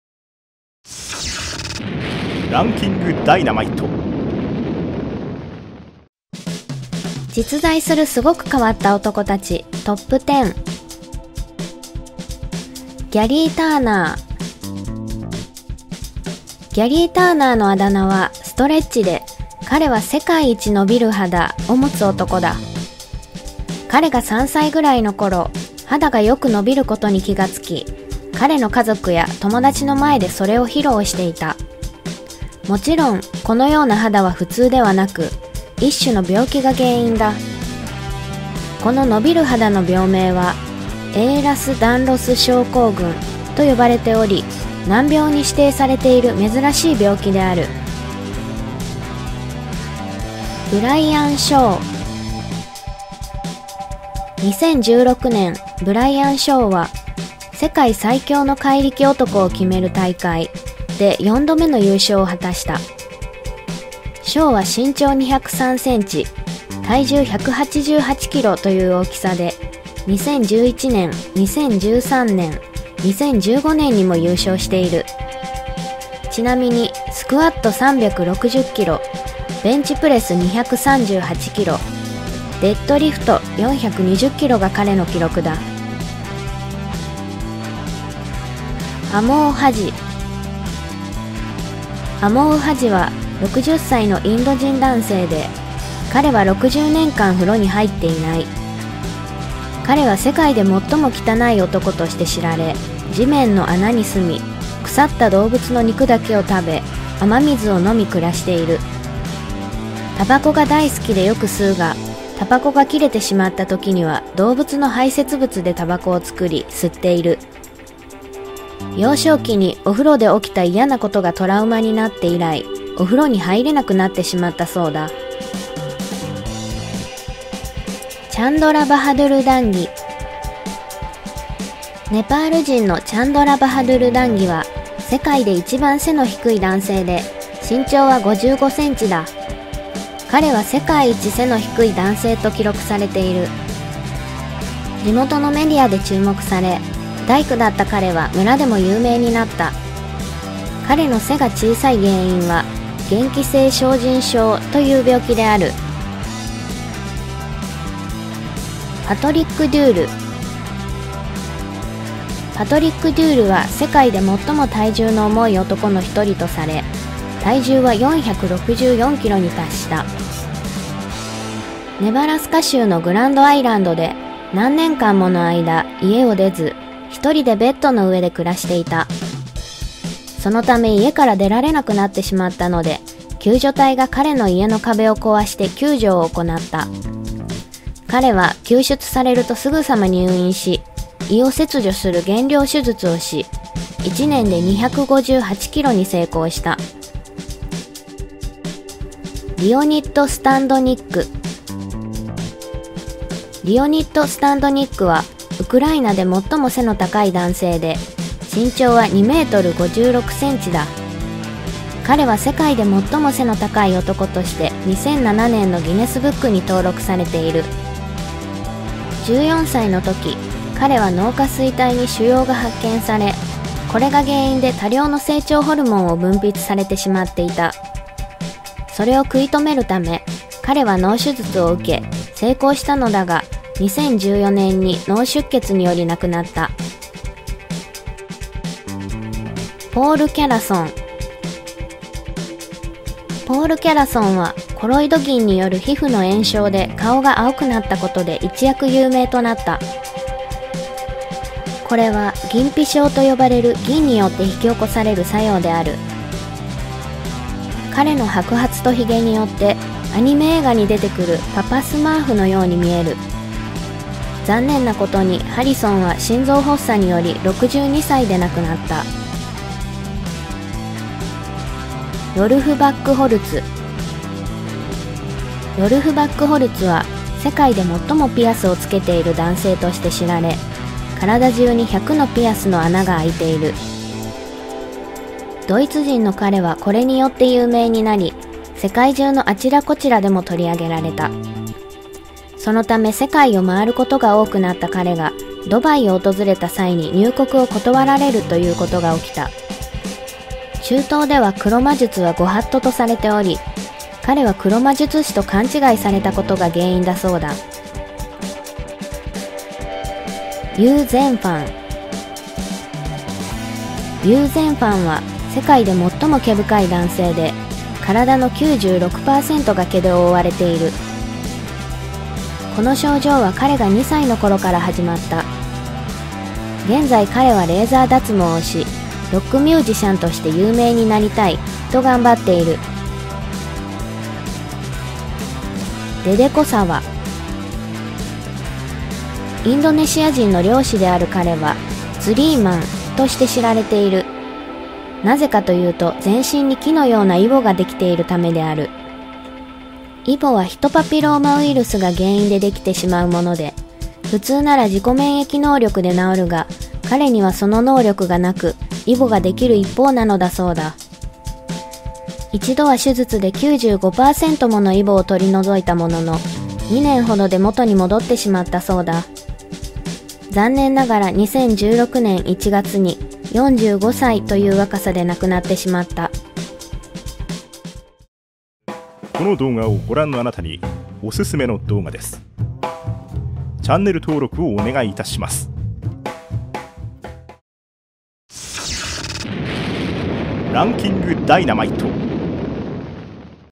実在する普通じゃない男たちＴＯＰ１０！世界最強の男から６０年風呂入らない男など、仰天事実に衝撃を受けること間違いなし！
★ランキングダイナマイト・公式ブログ